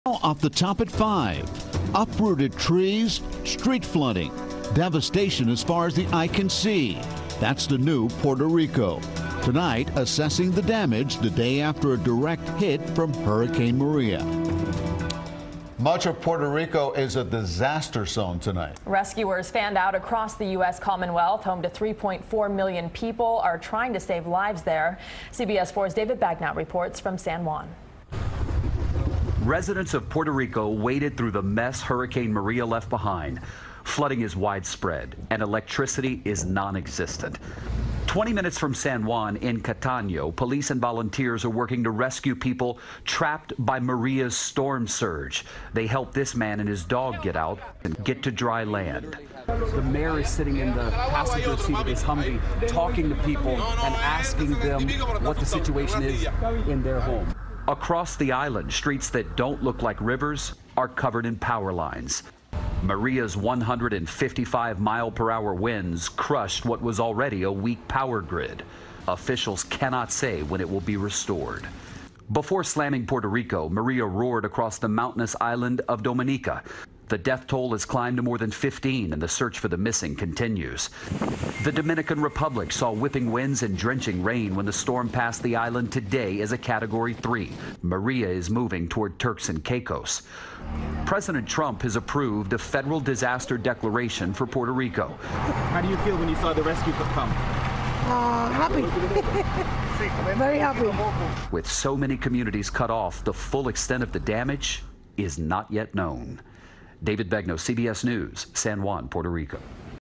Maria Left Path Of Destruction, No Power In Puerto Rico
CBS Miami's David Begnaud reports from Puerto Rico.